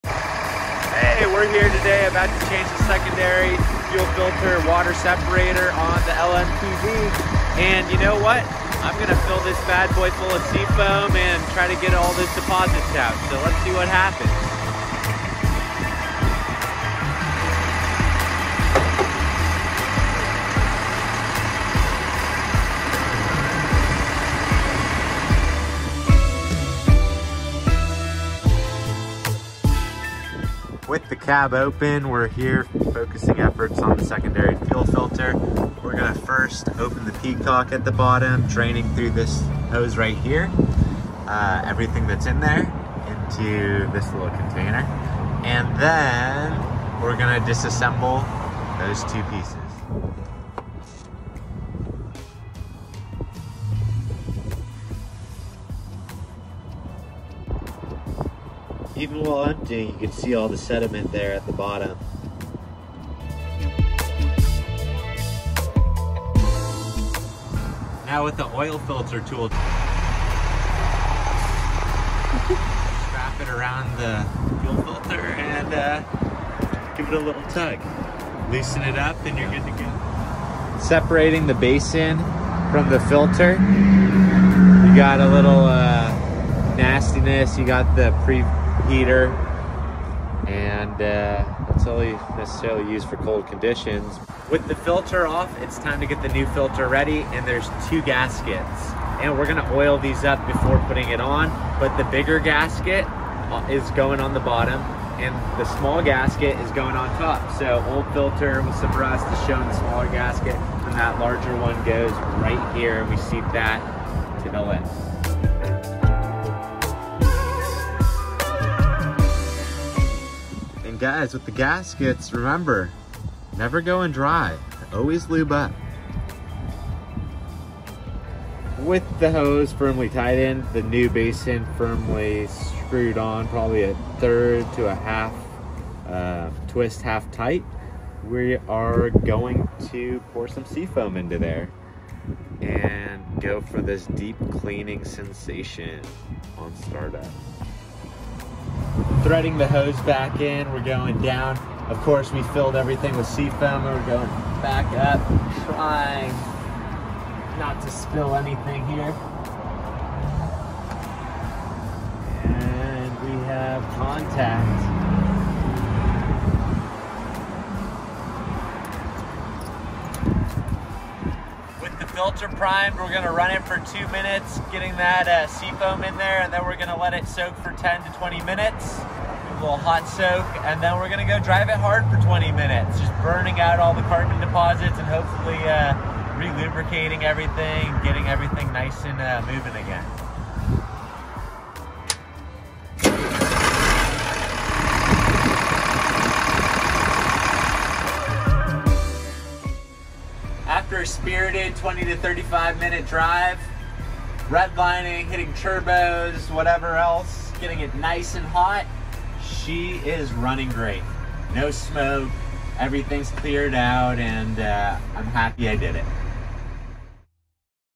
LMTV M1078 Secondary Fuel Filter / Water Separator Replacement
Is your 2.5 Ton+ LMTV running rough and blowing some smoke? Don't just change out the primary fuel filter... be sure to change out the secondary filter/water separator as weIl.

In the process, I pre filled the filter with Sea Foam to blast out all the carbon deposits and lubricate the fuel system. It's not a guaranteed fix but worked for me to clear up some unwanted smoke.

Filter Part #- R45P

*No Peacocks 🦚  were injured during the filming of this production. Enjoy!